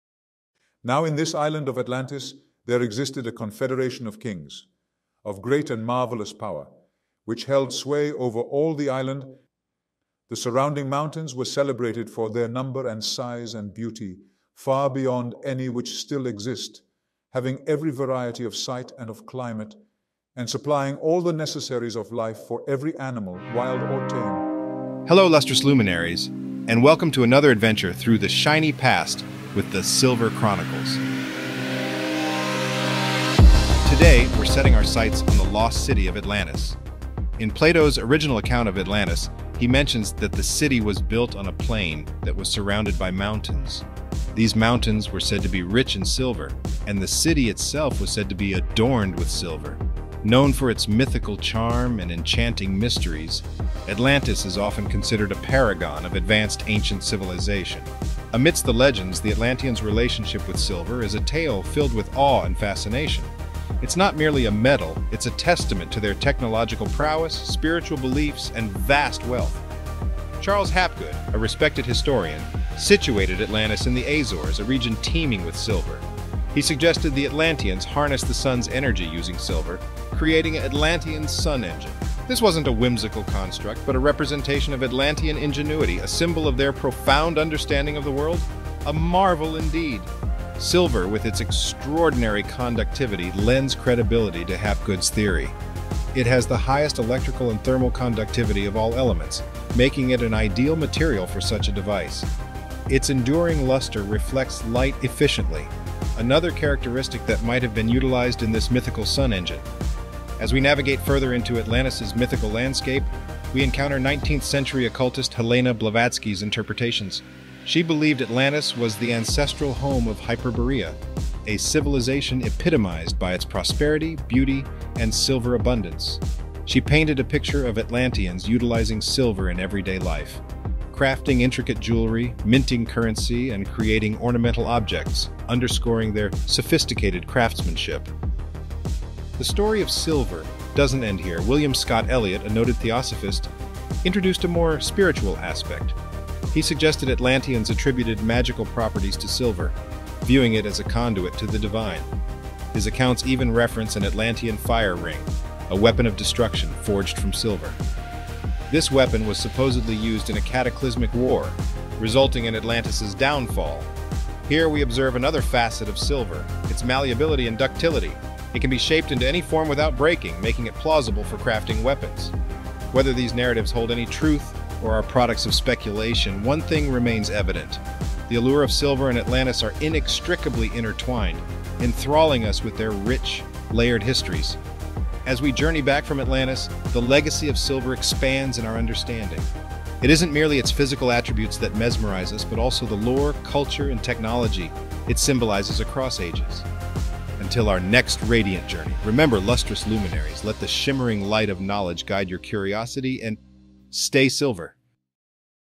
Atlantis' Silver Doom and Connections
Unlock the mystery of the legendary Atlantis with our newest deep-dive: "Silver's Atlantis Connection: The Lost Empire's Untold Treasure". Journey with us through Plato's silver-filled mountains to the speculated solar energy harnessing techniques of the Atlanteans. This video unveils theories of historians, geographers, and theosophists, shining light on the role of silver in Atlantis's technological advancements, spiritual practices, and potential warfare. Explore Atlantis's silver secrets, the Atlantean sun engine theory, and the myth of the destructive silver Atlantean Fire Ring. Stay curious, stay shining, stay silver!